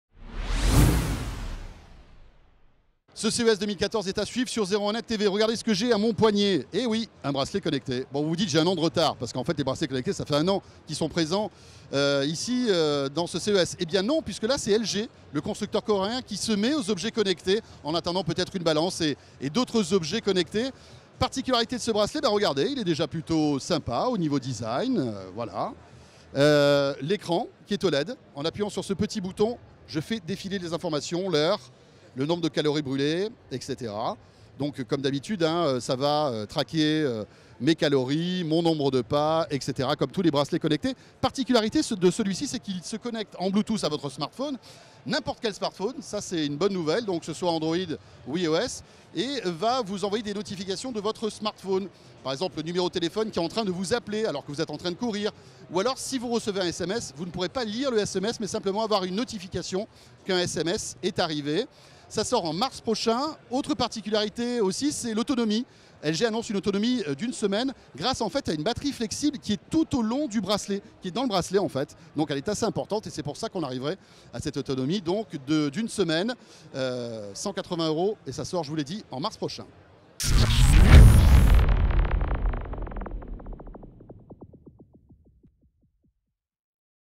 [CES14] : LG Lifeband Touch, un bracelet connecté qui sort du lot
Nous n'imaginions pas un instant que LG passerait à côté du phénomène des bracelets connectés. Et nous ne sommes pas trompés. Le géant Coréen présente actuellement le Lifeband Touch au CES de Las Vegas.  Côté design, difficile de parler d'innovation tant nous trouvons que sa ressemblance avec le Nike FuelBand est frappante.
De prime abord, le fonctionnement pourrait sembler classique. Le Lifeband Touch communique en Bluetooth avec un smartphone iOS ou Android sur lequel il affiche les informations habituelles : calories consommées, nombre de pas effectués, etc.  Fort de son expérience dans la téléphonie mobile et le multimédia, LG pousse plus loin le concept. Son bracelet affiche aussi les appels entrants, le nom morceau de musique que vous êtes en train d'écouter et  permet même d'en contrôler le volume.
Un partenaire endurant qui plus est. LG annonce 7 jours d'autonomie, grâce à une batterie flexible  placée dans tout le bracelet. Le Lifeband Touch sera disponible en mars au prix de 180€.

: technologie, nouveautés, Smartphones, tablettes, ordinateurs, innovation, Web,  numérique, informatique